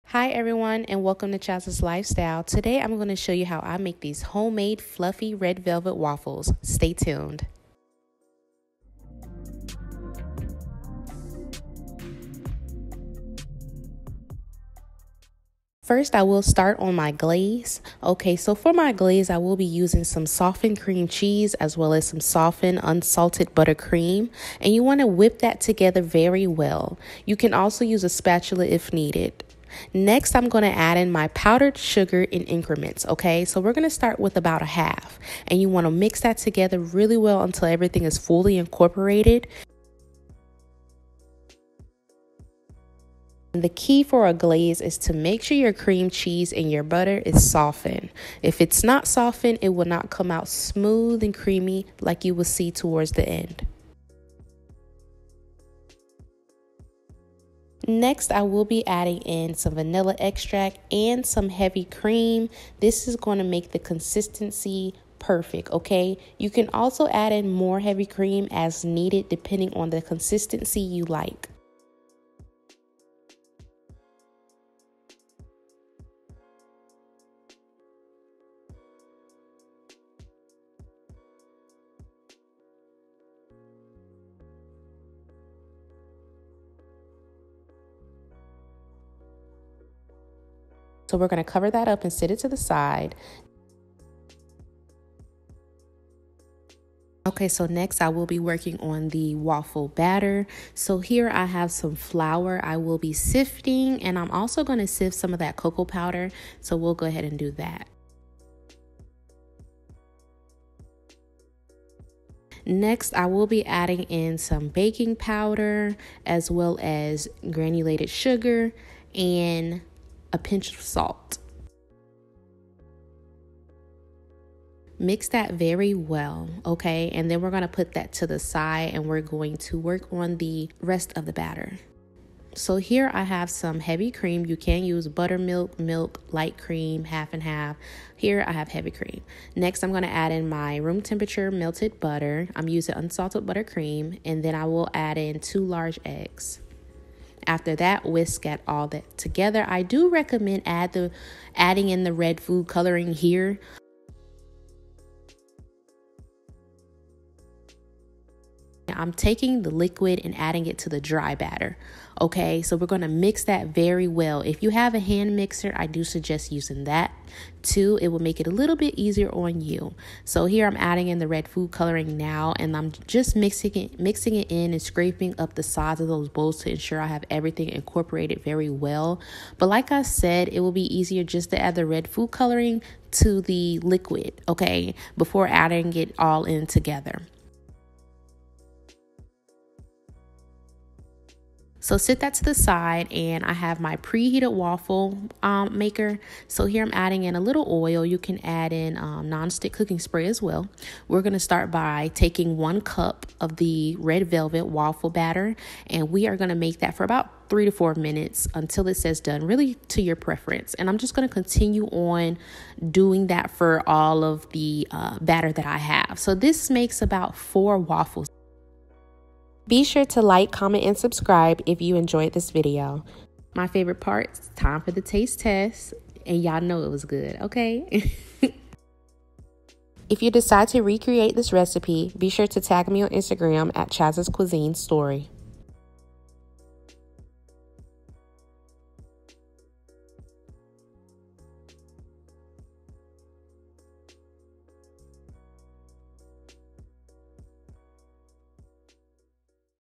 Fluffy Red Velvet Waffles Recipe | Valentine’s Day Ideas | Chaz’s Cuisines
Hi, everyone!
 Thank you so much for tuning in. I hope you're here to stay and connect!

2 Cups all-purpose flour
¼ cup, sugar
½ tsp kosher salt
2 tbsps cocoa powder
1 tbsp baking powder
⅓ cup, melted unsalted butter cream( cool before using)
1 ¾ cups, light cream
2 large eggs
2 tbsps, red food coloring
2 tsp vanilla extract

Icing/Glaze:
4 tbsps, unsalted softened butter
4 oz softened cream cheese
1 tsp vanilla extract
½ cup, light cream
1 ½ cups, confectioners sugar